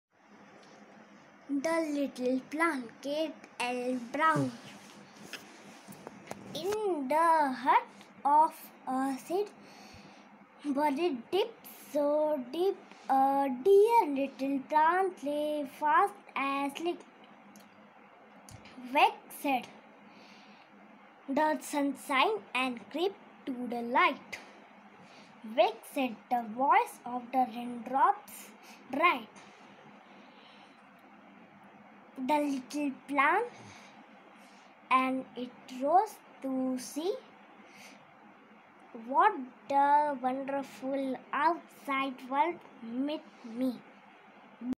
The Little Plant by K. L. Brown recited by Vatsal Chatterjee
poem for class 1 ICSE